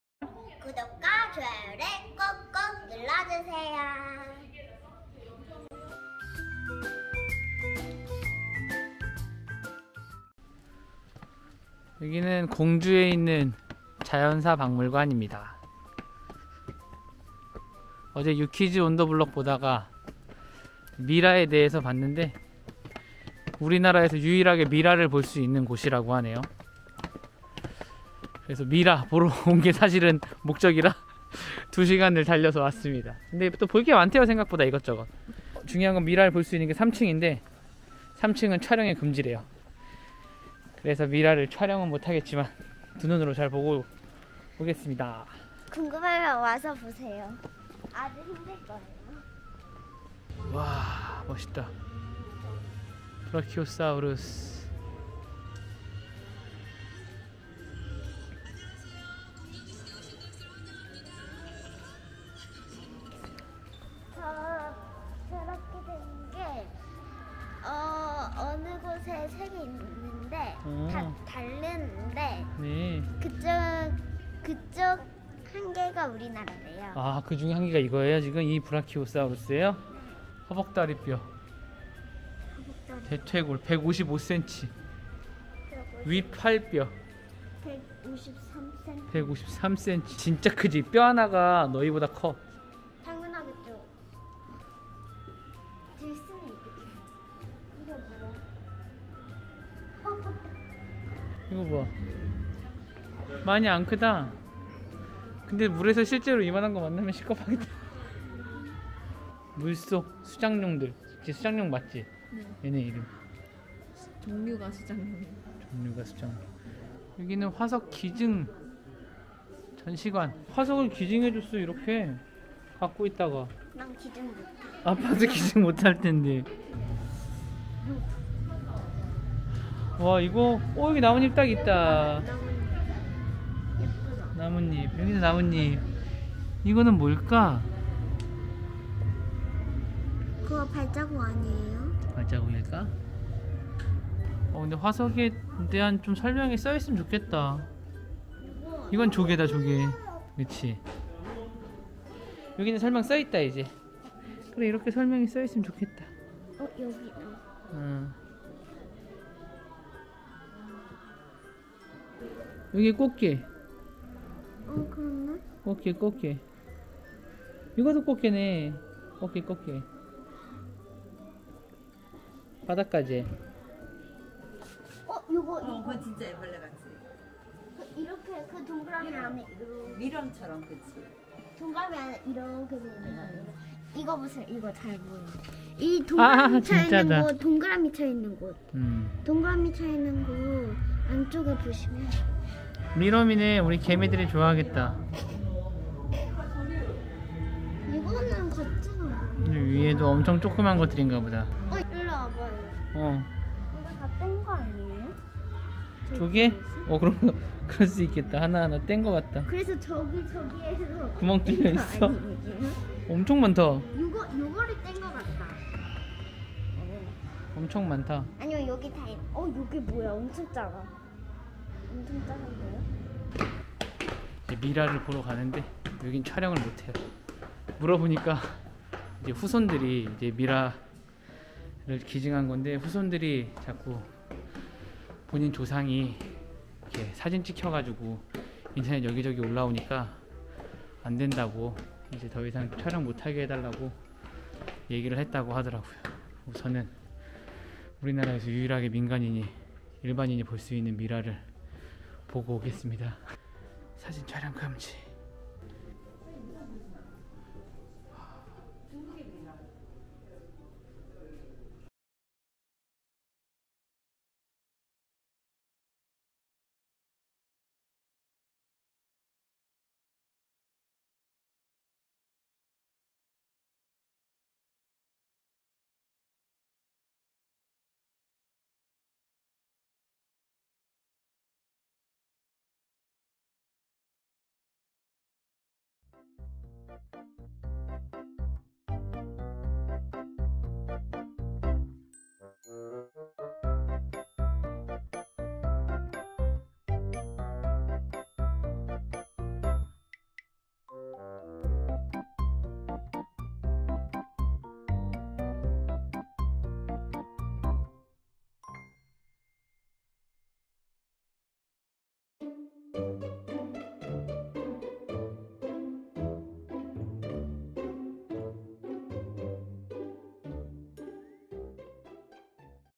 [브이로그]한국자연사박물관에 미라를 보러 가다!!
한국에 미라를 볼 수 있는 곳이 있대요.
바로 한국자연사박물관!
예배를 마치고 2시간을 달려서 공주까지 갔습니다
음..미라는 생각보다 적나라하고..무서웠지만 그래도 한번쯤을 볼만한 경험이었어요.
드림하이가 잘 봐줘서 고맙ㅎ
그리고 서울올라오는 길은 3시간30분ㅋㅋ